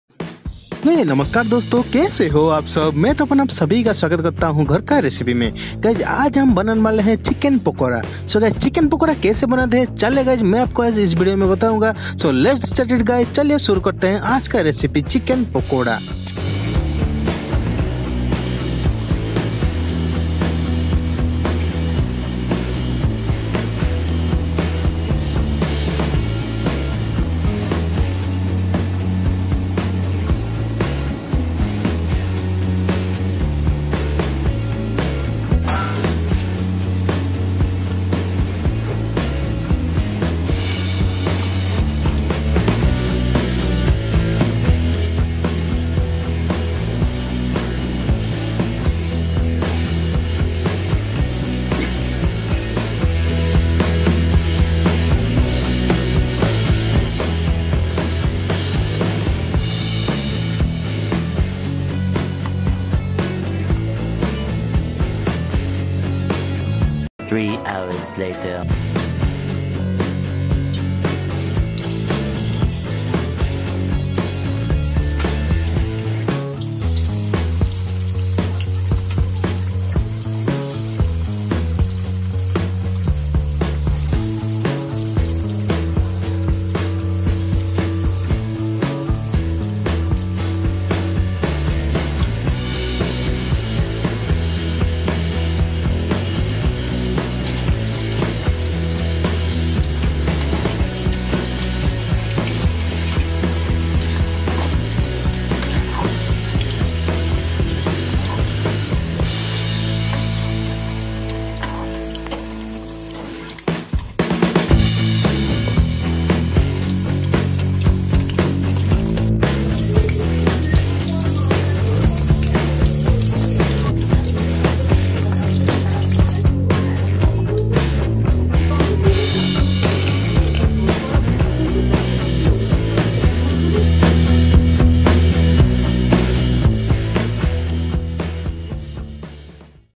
Crispy chicken Pakora Recipe 🍗 How to Cook Crispy Chicken Pakora street style || ghar ka Recipe
Guy'saj hum banane bale he Crispy Chicken Pakora. So guy's kaise Banta he chaliye dekhte hain.Chicken pakora are delicious crispy chicken fritters, loaded with spice which give these crispy bite-sized snacks/appetizers lot of amazing flavors. If you like any kind of fried chicken then I urge you to try this spectacular Indian dish.

NOTE :All content used is copyright to 'Goo News'